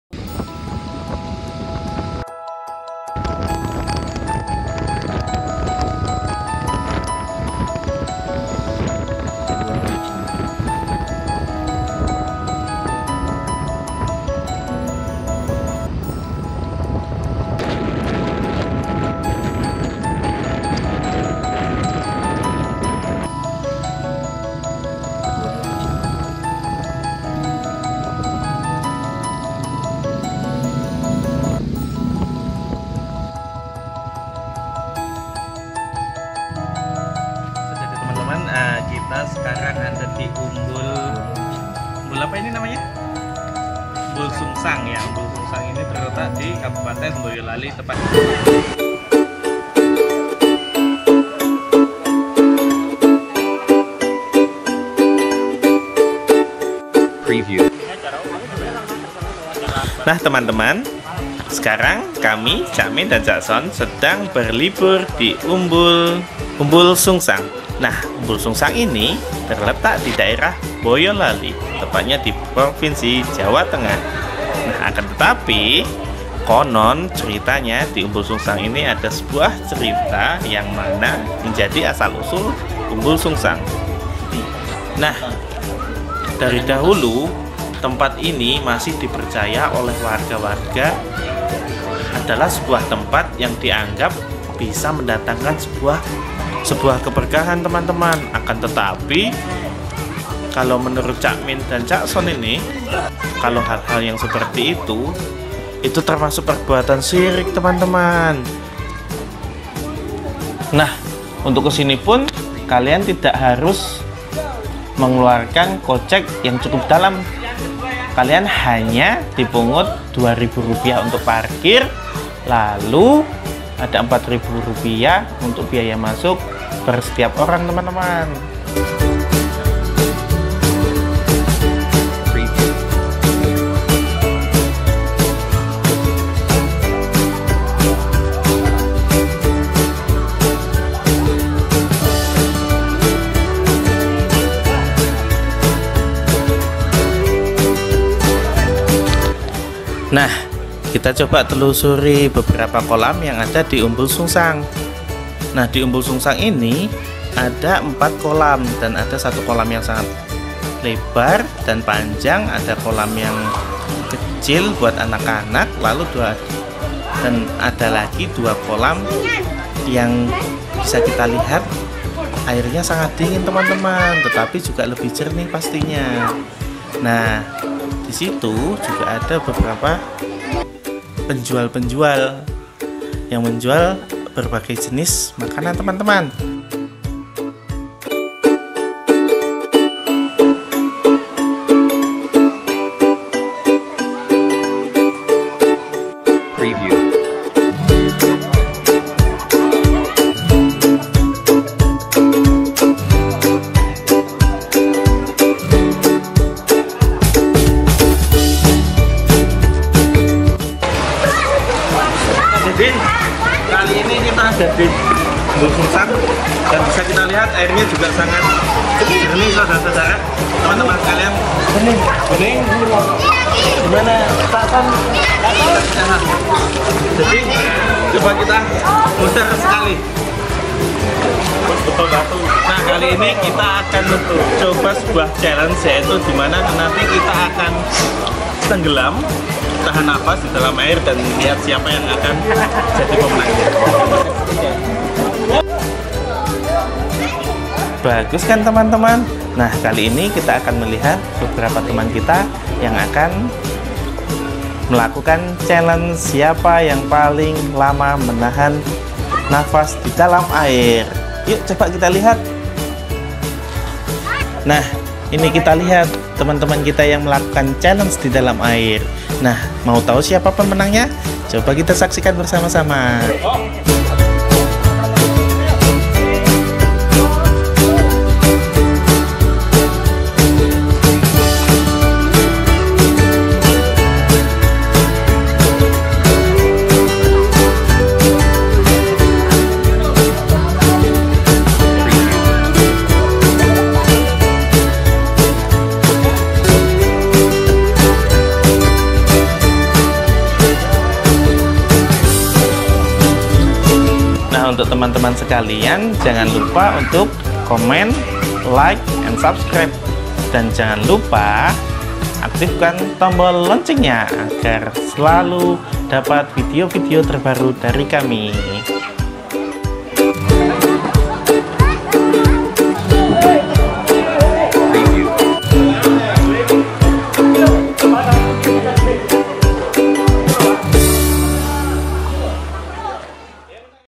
Review Umbul Sungsang ll wisata pemandian boyolali
Assalamualaikum sahabat lampu hijau channel, pada video kali ini kami cak min dan cak son akan mereview tentang pemandian atau umbul yang dikenal dengan nama umbul sungsang. Wisata pemandian dengan kantong yang ringan, dan pesona tempat yang alami. Mari tonton video kami, wisata pemandian umbul sungsang boyolali.

Follow juga IG kami
cak min : Wendi_wiranata_kismadi
cak son : ahmad_khoirudeen